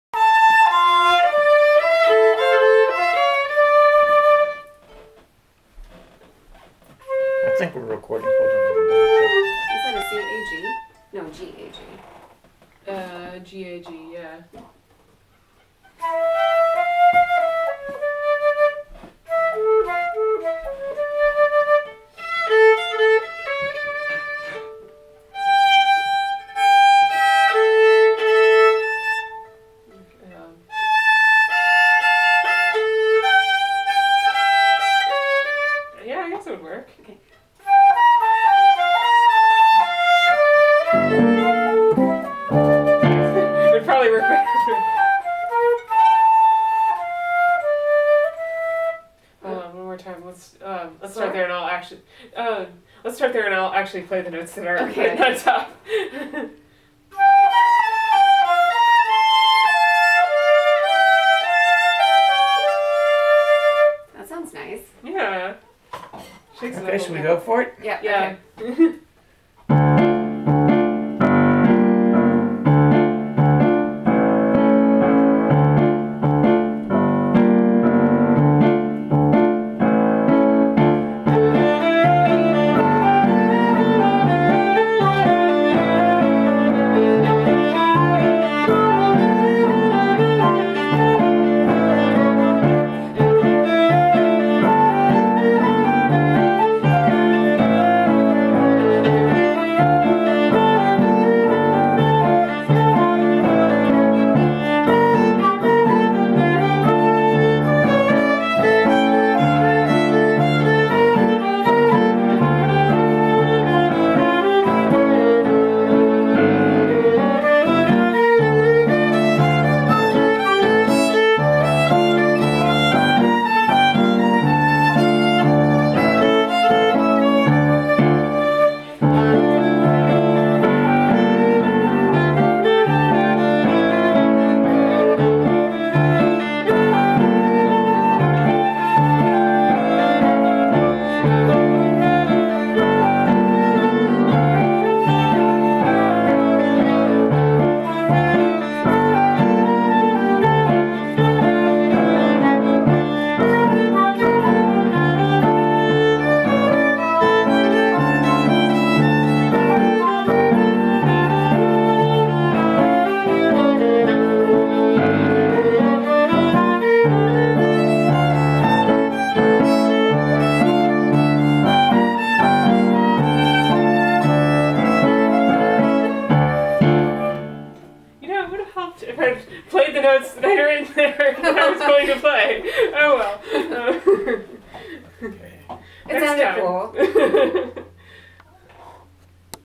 !!!2021 07 18 Sylvi
Return to normalcy after a long separation due to COVID-19.  A Celtic Jam session with some talented musician friends.  I left the interactions between musician in to demonstrate the importance of rehearsing live, to work out nuances prior to the performance.